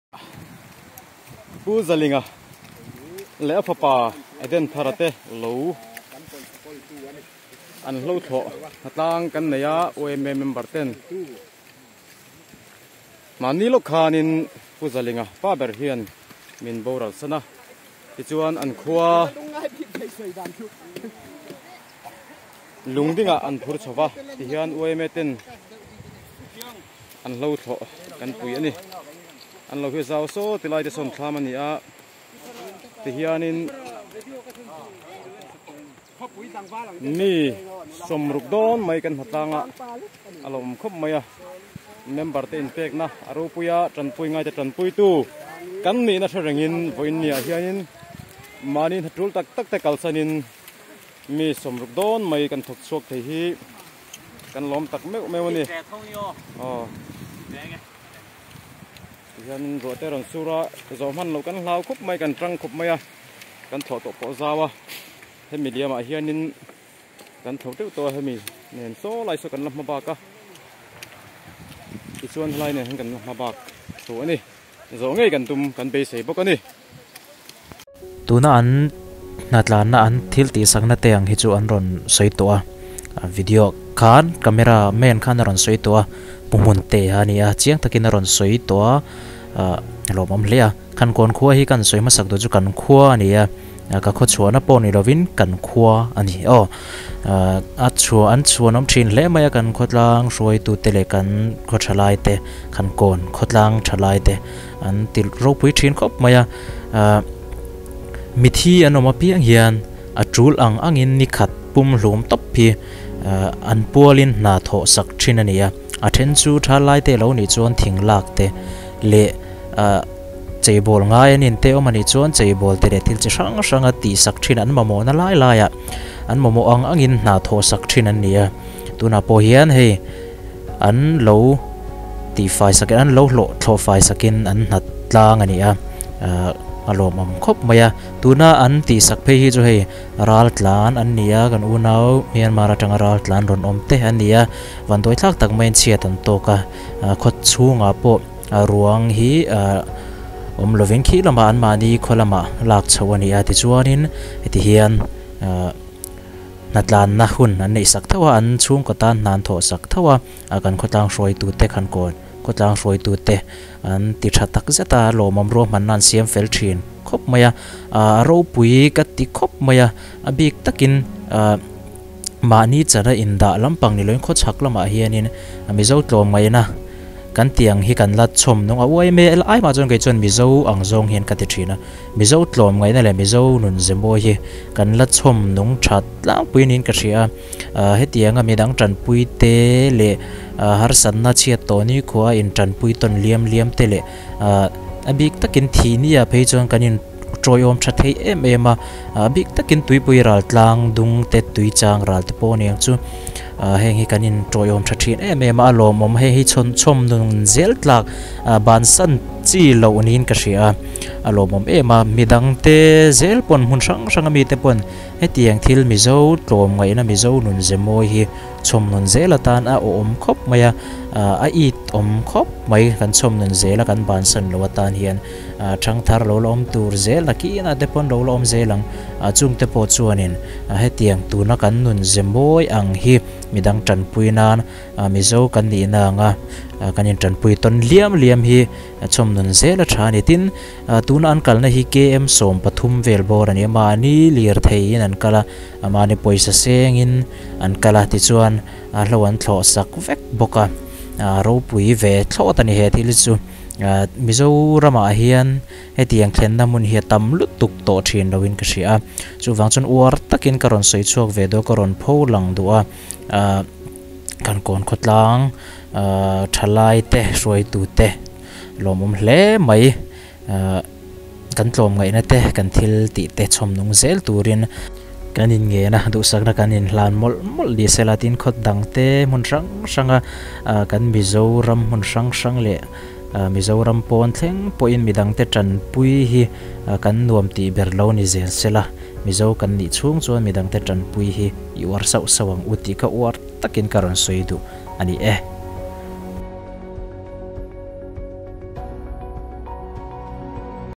khankawn a thil thleng!! Avahan ropui tehreng em..
khankawn hi mizoram khawchhak Champhai district Farkawn bul mai a awm ani